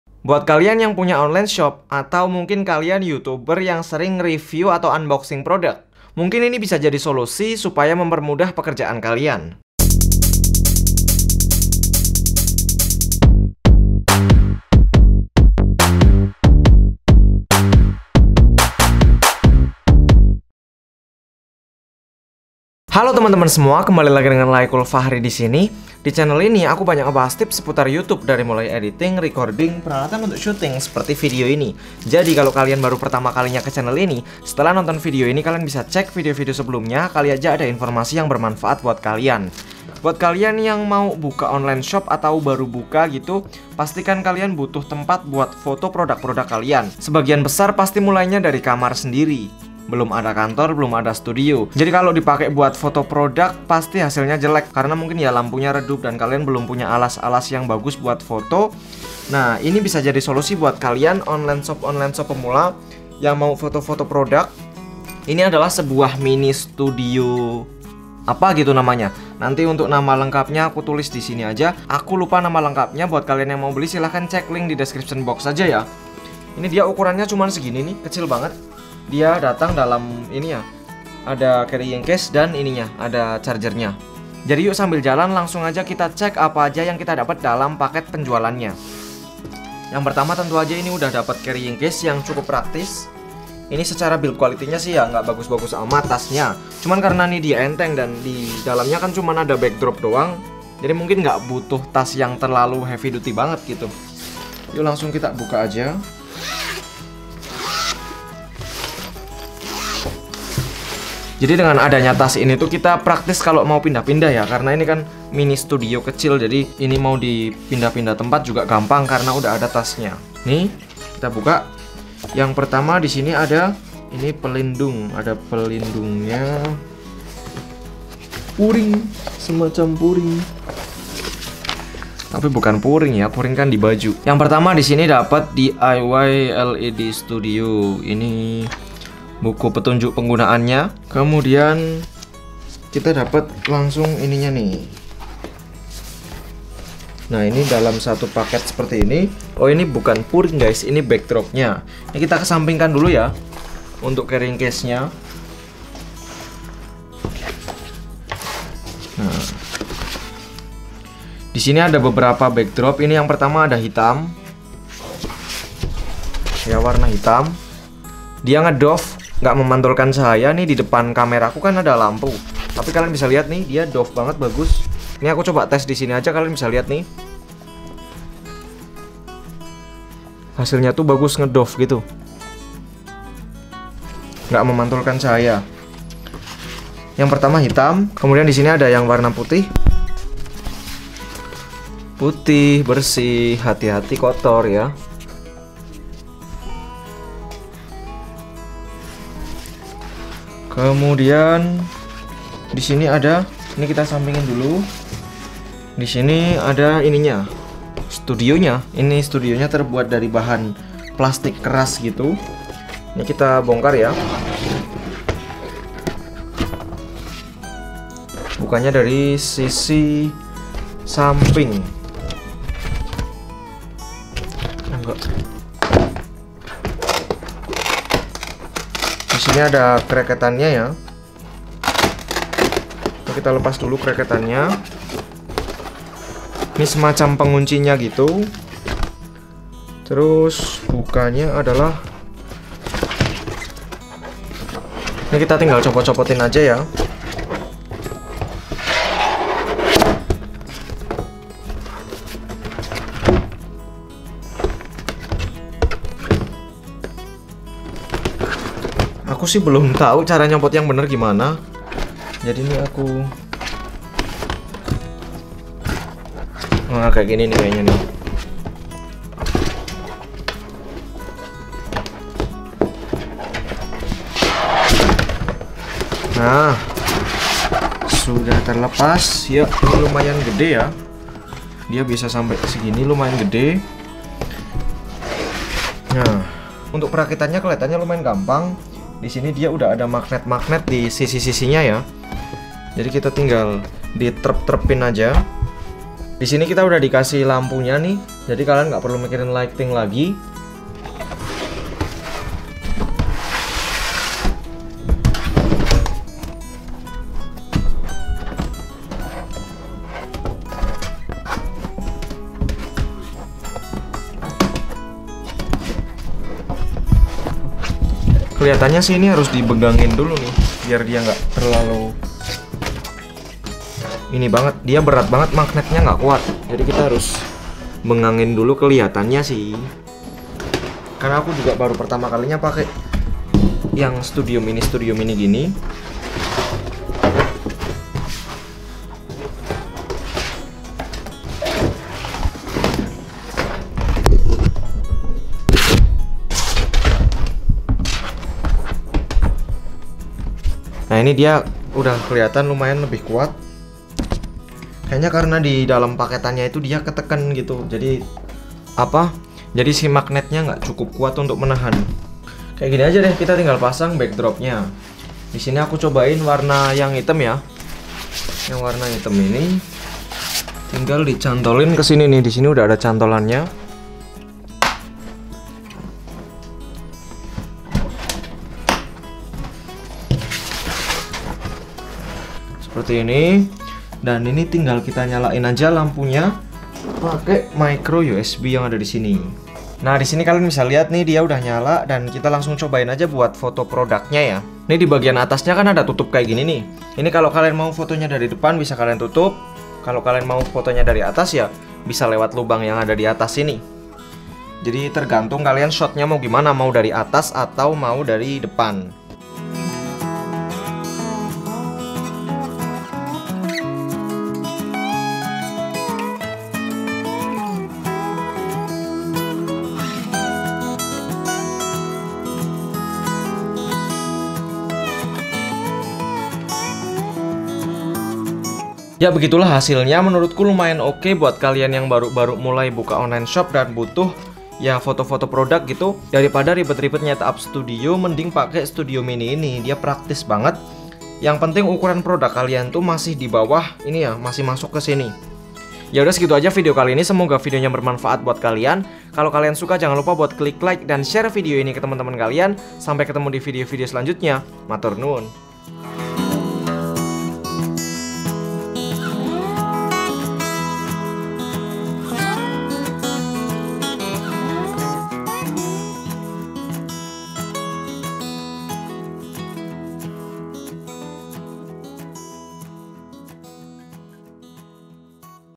pengen punya bisnis online ? kesulitan foto produk ? photo studio box mini olshop makin lancar
Unboxing & Review Mini Studio Box